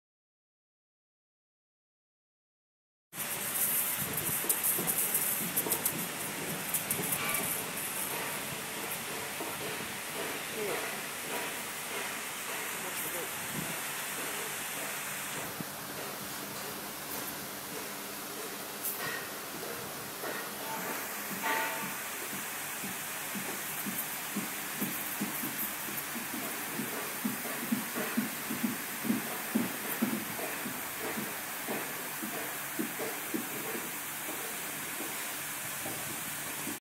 Splashpoint Worthing
Splashpoint at Worthing Seafront featuring a misting water feature by Ustigate